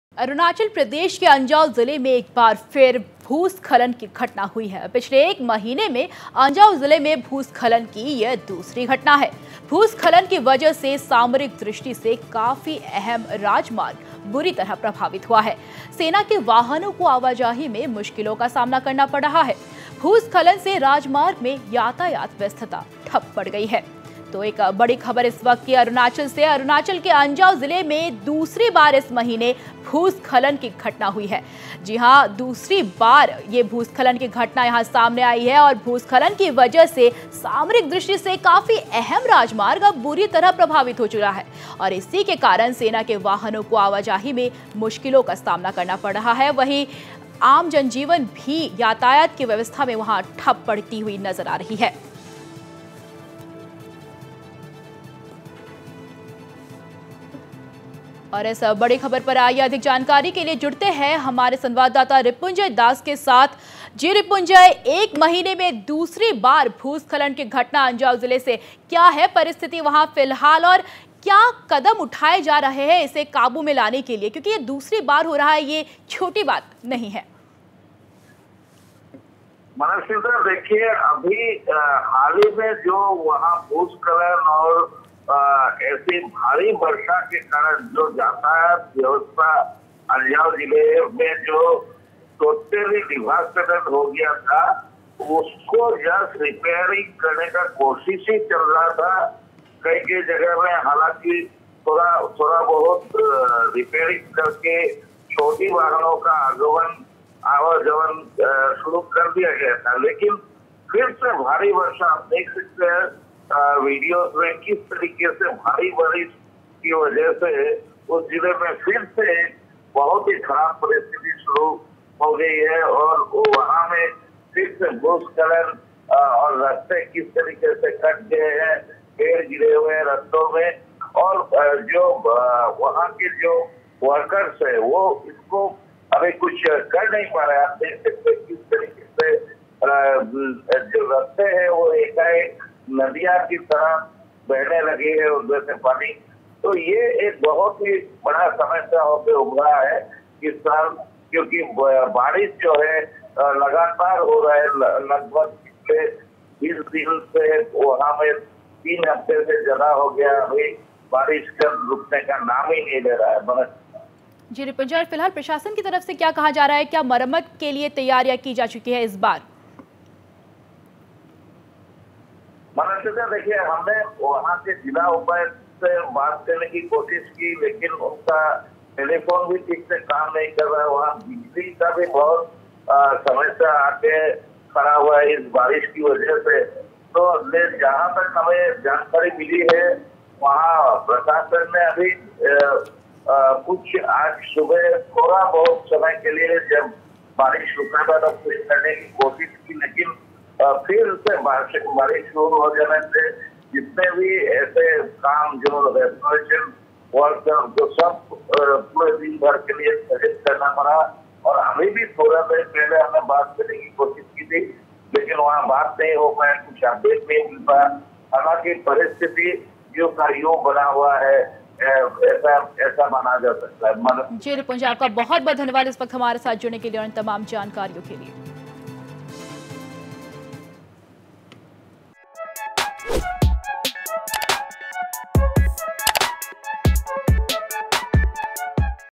अरूणाचल के अंजाओ में भू-स्खलन, यातायात व्यवस्था ठप
ArunachalPradesh | अरूणाचल प्रदेश के अंजाओ जिले में एक बार फिर भू-स्खलन की घटना हुई है। पिछले एक महीने में अंजाओं जिले में भूस्खलन की यह दूसरी घटना है। भूस्खलन की वजह से सामरिक दृष्टि से काफी अहम राजमार्ग बुरी तरह प्रभावित हुआ है। सेना के वाहनों को आवाजाही में मुशकीलों का सामना करना पड़ रहा है। भूस्खलन से राजमार्ग में यातायात व्यवस्था ठप पड़ गई है।